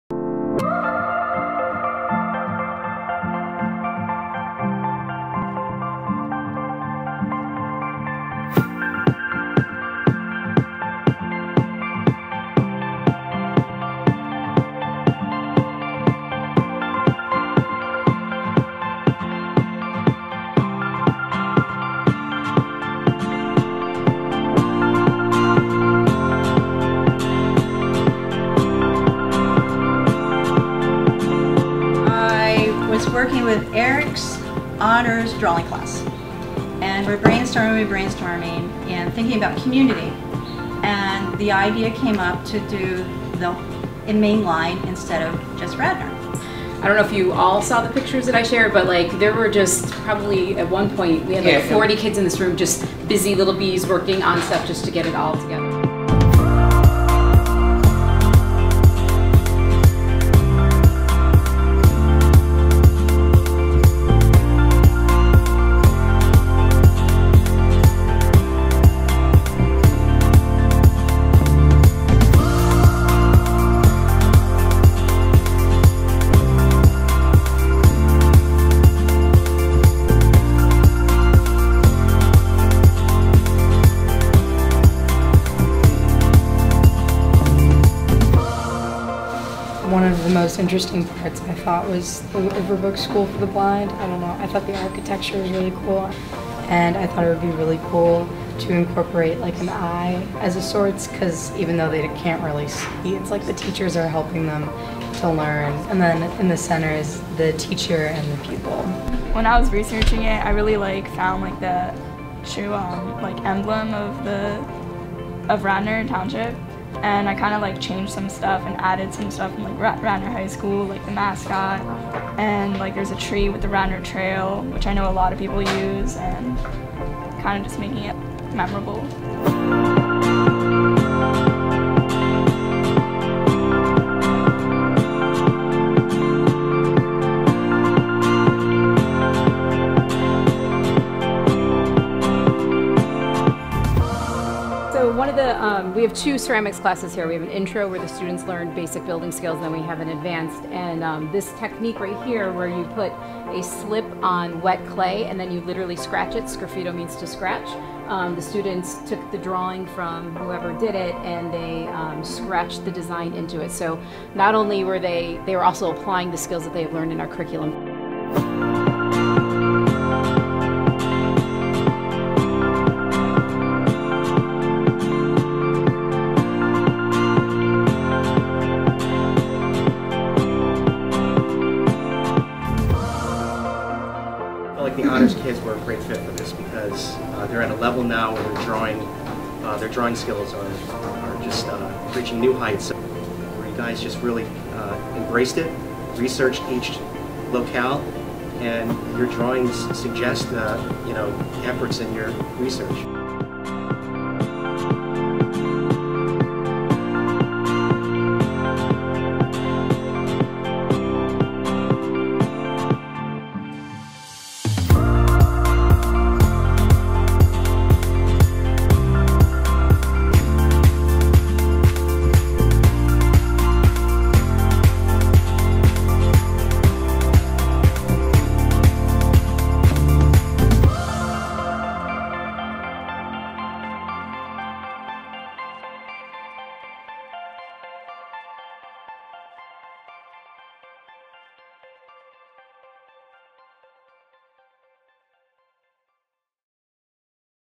RHS Art Students Featured at KOP Plaza
Radnor High School art students art classes worked with resident teaching artist Baily Cypress to design and create an original mosaic for installation at the King of Prussia Service Plaza on the Pennsylvania Turnpike. The project is part of Art Sparks, an effort by the Pennsylvania Turnpike Commission (PTC) and the Pennsylvania Council on the Arts (PCA) to bring student-created art to service plazas across the PTC's 550-mile system.

RHS's mixed media mosaic's 17 medallions represent the communities of the Main Line that surround the King of Prussia Service Plaza, each depicting a landmark selected by the students on the basis of its community, historic or aesthetic merit.